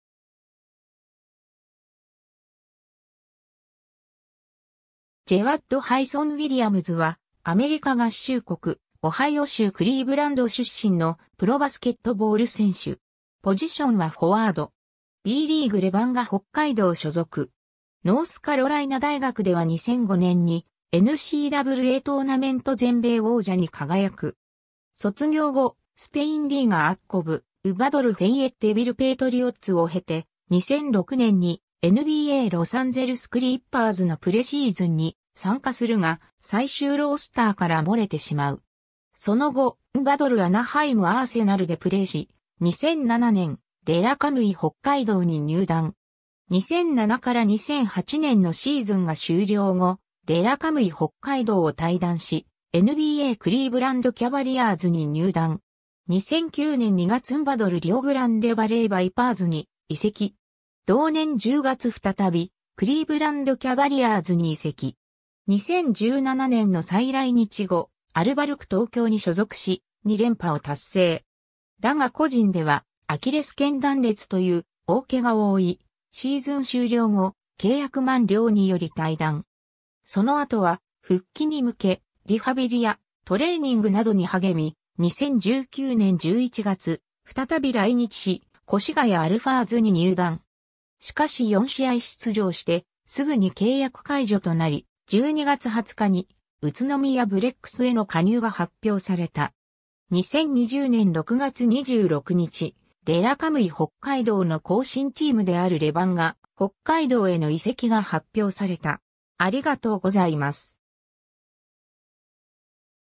ジェワッド・ウィリアムズ
リオグランデバレー・バイパーズの選手
ジェワッド・ハイソン・ウィリアムズ（Jawad Hason Williams、1983年2月19日 - ）は、アメリカ合衆国・オハイオ州クリーブランド出身のプロバスケットボール選手。
 Bリーグ　レバンガ北海道所属。
 ノースカロライナ大学では2005年にNCAAトーナメント全米王者に輝く。
 卒業後、スペインリーガACB、NBADLフェイエッテビル・ペイトリオッツを経て、2006年にNBAロサンゼルス・クリッパーズのプレシーズンに参加するが、最終ロースターから漏れてしまう。
 その後、NBADLアナハイム・アーセナルでプレーし、2007年レラカムイ北海道に入団。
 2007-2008年のシーズンが終了後、レラカムイ北海道を退団し、NBAクリーブランド・キャバリアーズに入団。
 2009年2月NBADLリオグランデバレー・バイパーズに移籍。
同年10月再びクリーブランド・キャバリアーズに移籍。
 2017年の再来日後、アルバルク東京に所属し、2連覇を達成。
だが個人では、アキレス腱断裂という大怪我を負い、シーズン終了後、契約満了により退団。
 その後は復帰に向け、リハビリやトレーニングなどに励み、2019年11月、再び来日し、越谷アルファーズに入団。
しかし4試合出場してすぐに契約解除となり、12月20日に宇都宮ブレックスへの加入が発表された。
 2020年6月26日、レラカムイ北海道の後身チームであるレバンガ北海道への移籍が発表された。